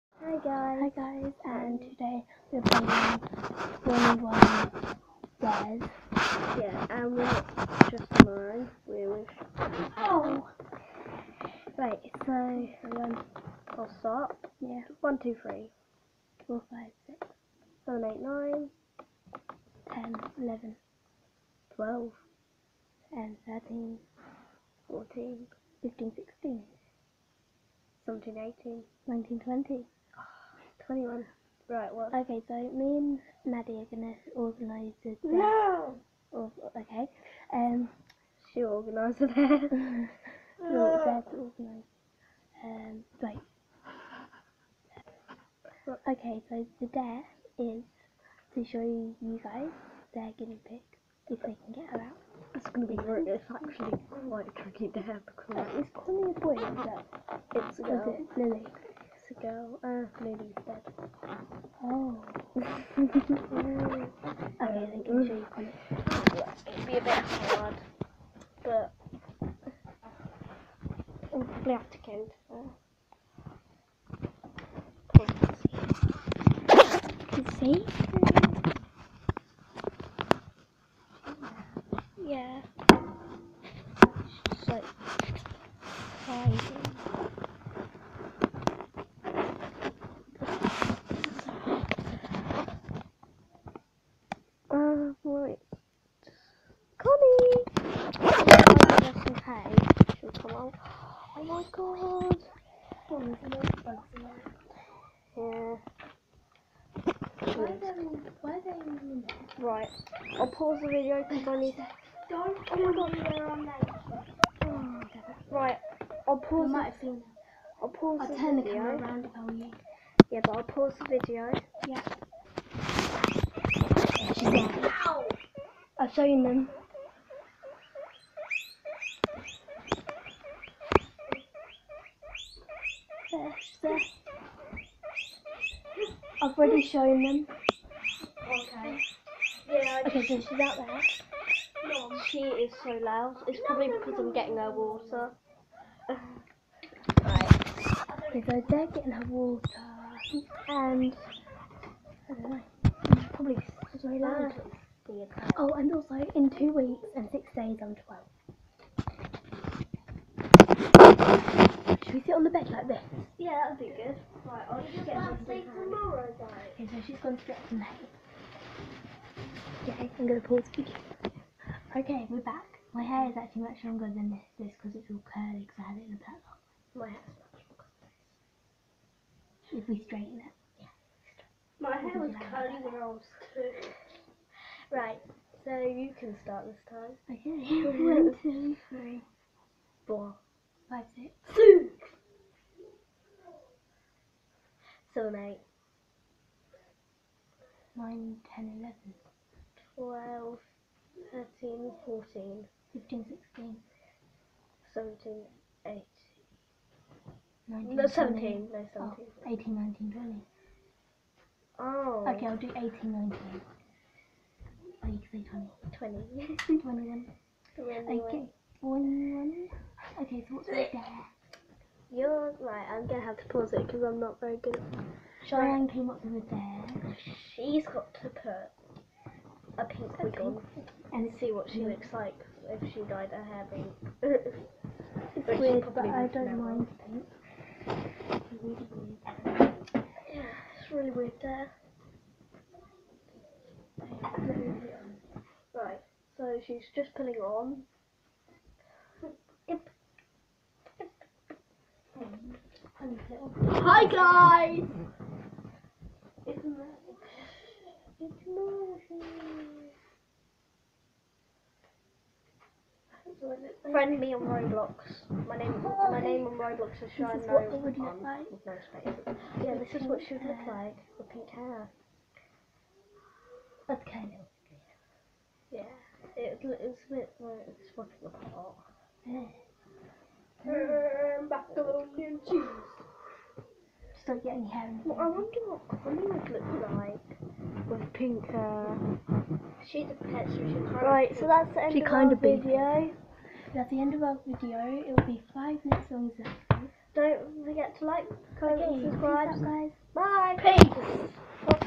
21 dares w/ charlotte
We have a dare each plus with a special guest!!!